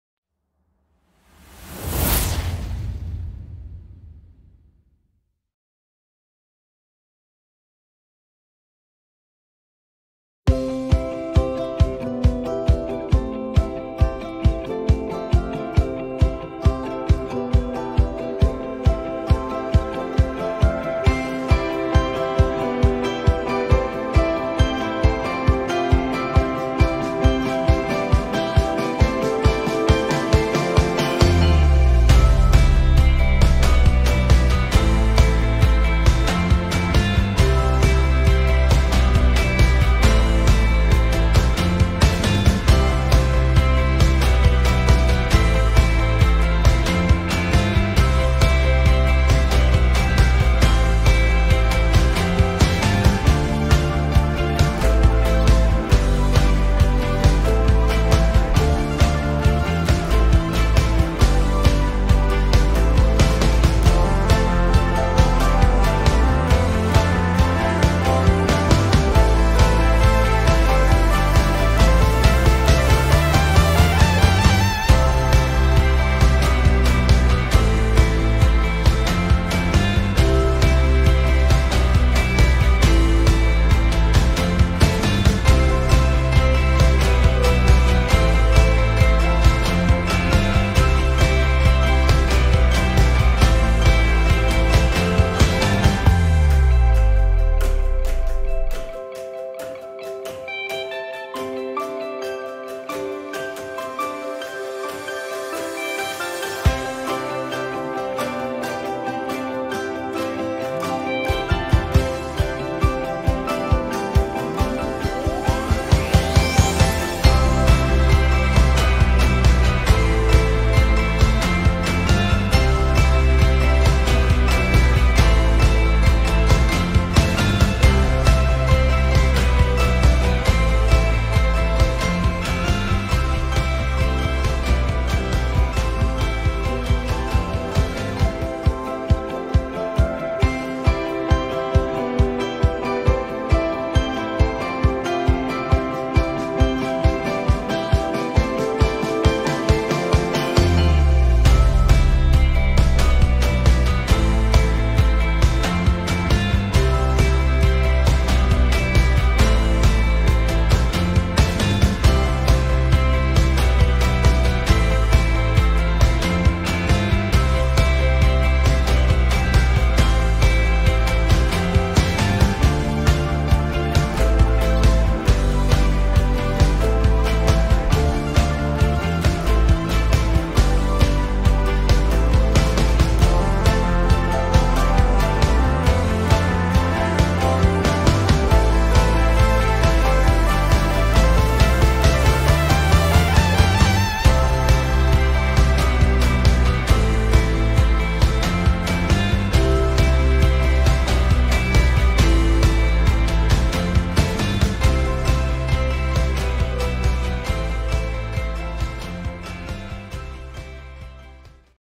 Th-THAILANDIA - Cambio Euro Bath a PATONG - PHUKET- 4k
Arrivare in una città nuova potrebbe comportare problematiche relative alla moneta che si deve utilizzare per la propria quotidianità , con questo video cerco di introdurvi con meno stress possibile su come poter arginare questa situazione cambiando la propria moneta senza problema alcuno!

Spero questo mio video  vi sia utile Amici!

 Attivate la campanellina per non perdervi  ogni contributo ! 😊
Un mezzo di sostentamento ed uno stimolo per tutti i creator!